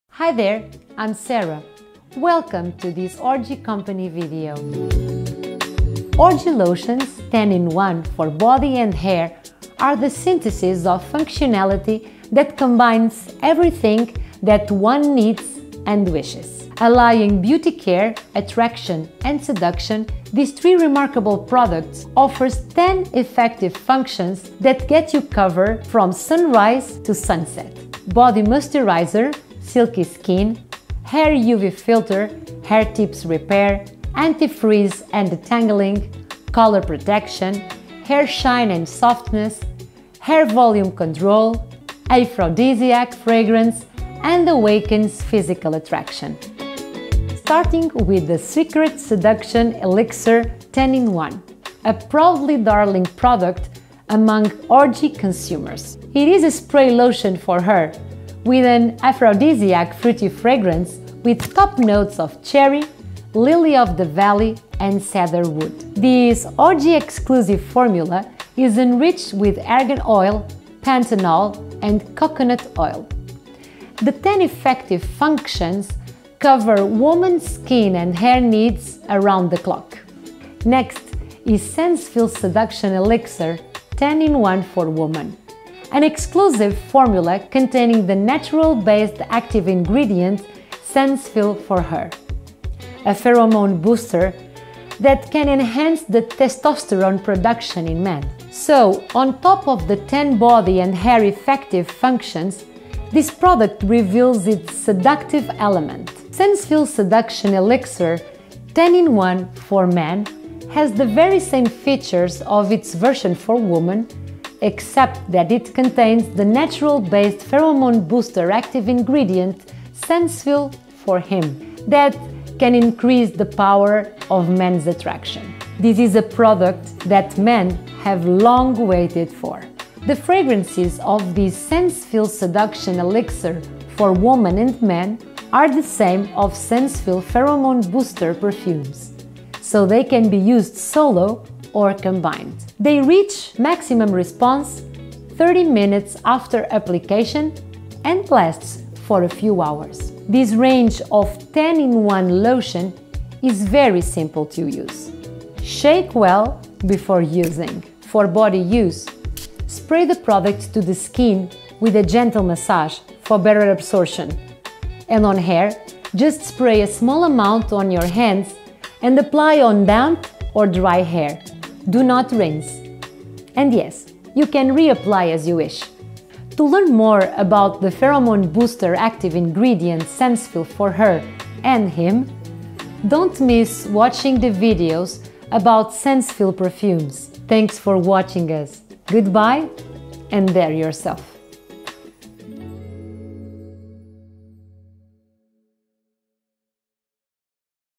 Orgie 10 in 1 Hair and Body Lotions - English
Orgie Company's host explains all about the product and how to use.